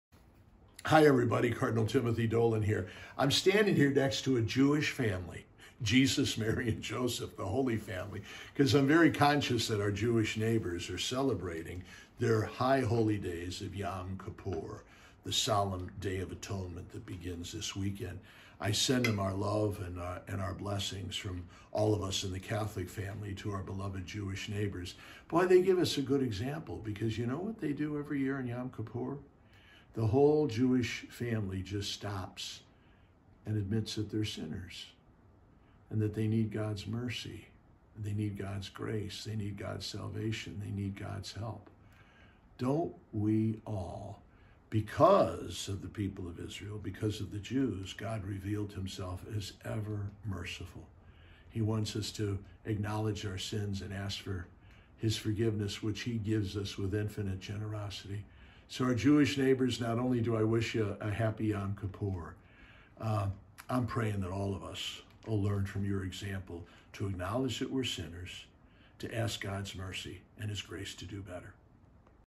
Yom Kippur
​Our Jewish neighbors are celebrating Yom Kippur this weekend. It is a day to recognize they need God's grace and mercy. Don't we all? I'm praying we all learn from the example of our Jewish neighbors ​to acknowledge we are sinners that need God's help to do better.